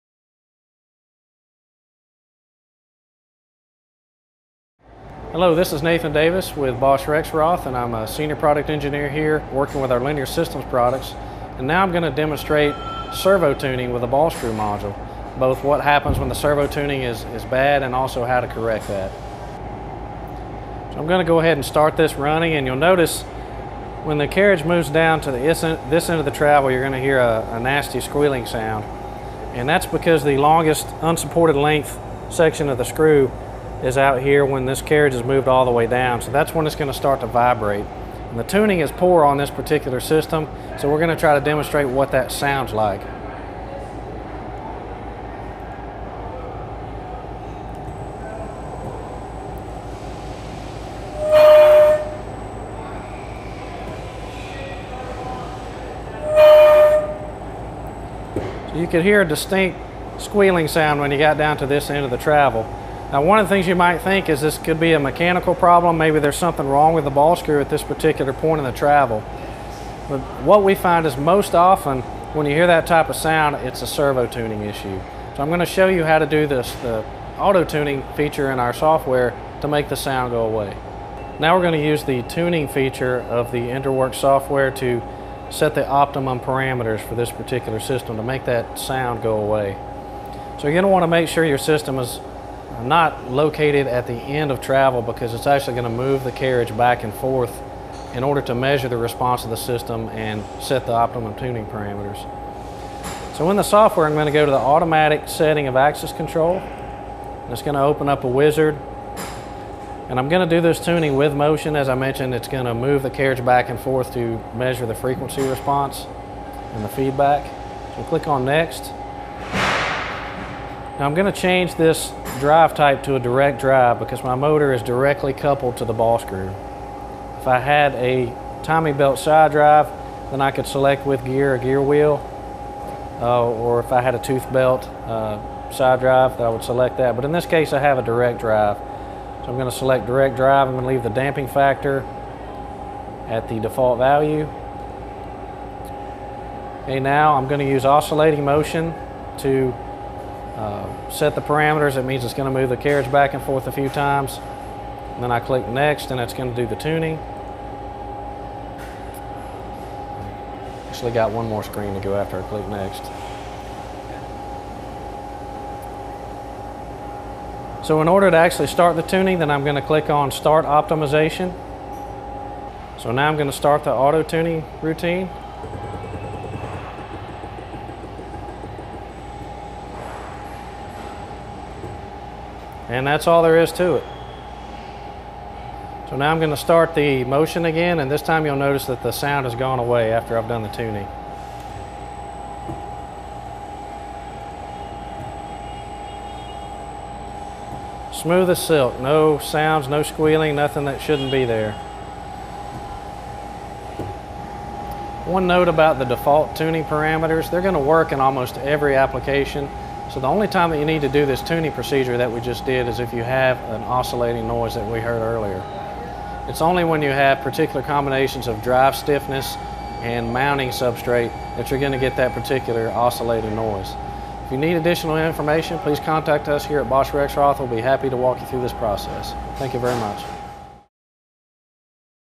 Ball Screw Module Servo Tuning
As a result of certain combinations of drive stiffness and mounting substrate, ball-screw driven linear modules may occasionally exhibit oscillation noise if not properly tuned. This video gives step-by-step instructions on how to use the IndraWorks software package auto-tuning feature to set the optimum tuning parameters for ball-screw-driven linear modules.